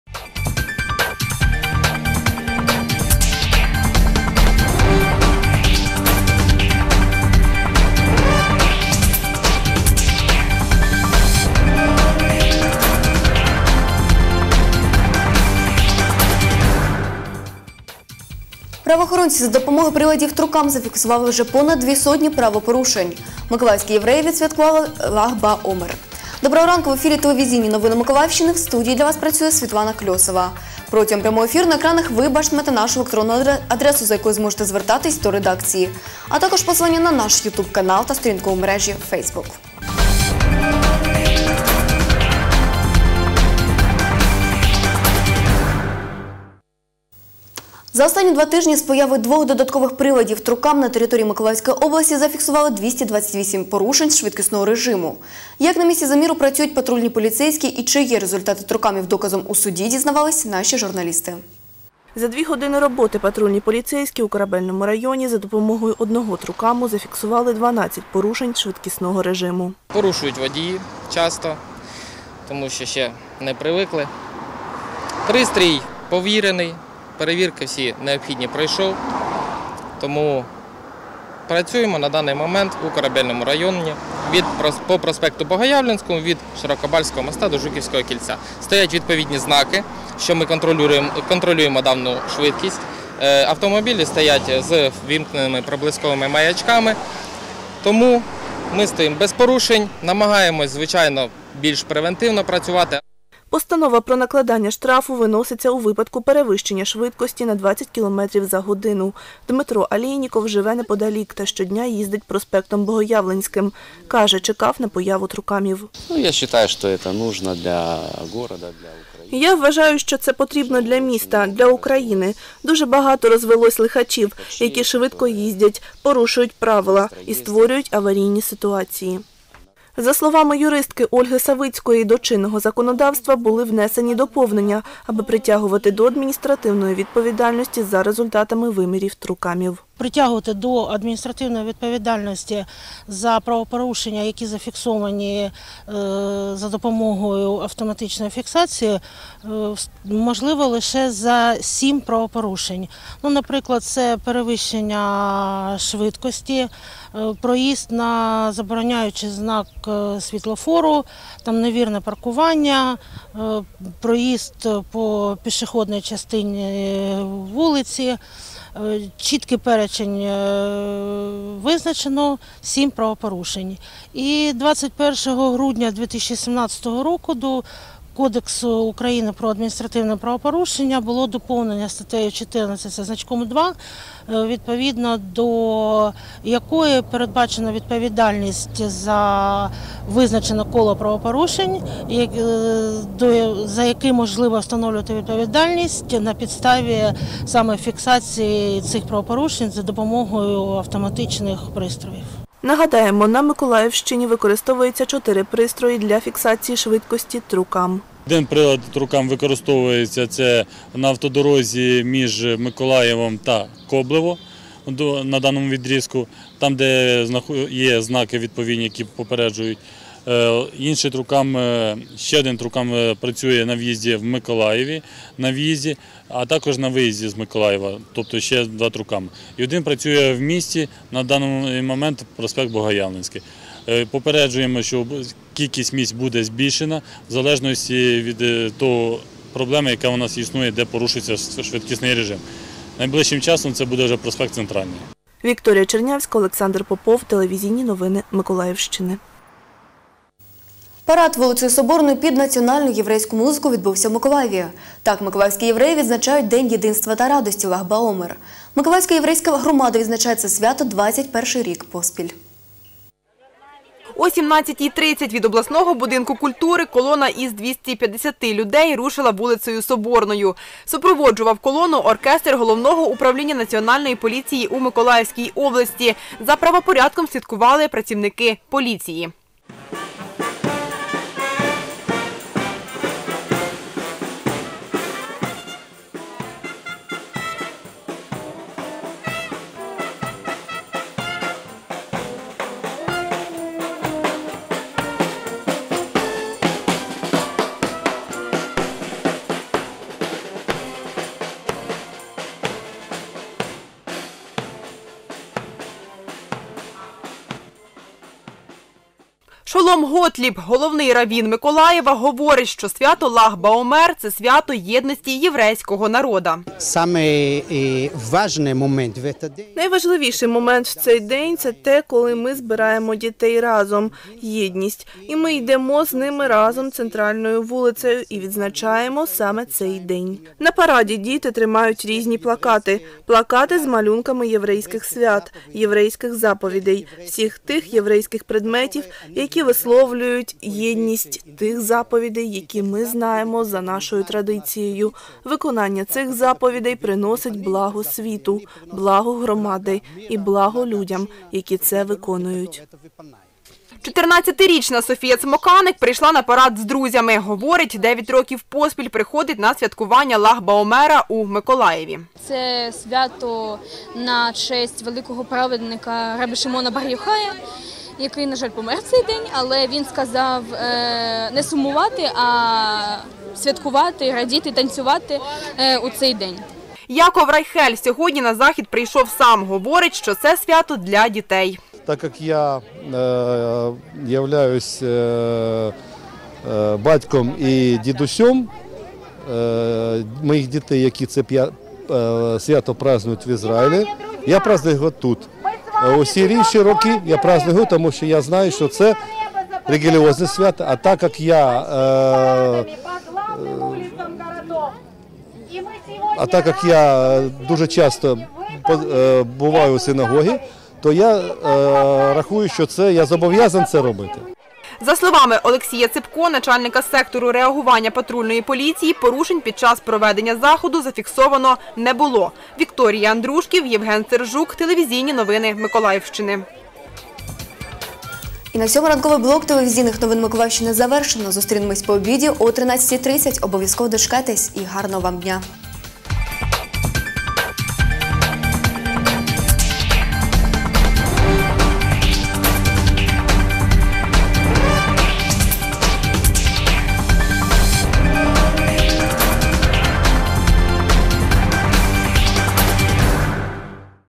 24.05.2019. Новини. 08:30
Інформаційна картина дня (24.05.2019) поглядом кореспондентів телеканалу “Миколаїв” (випуск новин —08:30):
- Правоохоронці за допомогою приладів Трукам зафіксували вже понад дві сотні правопорушень
- Миколаївські євреї відсвяткували Лаг-ба-Омер.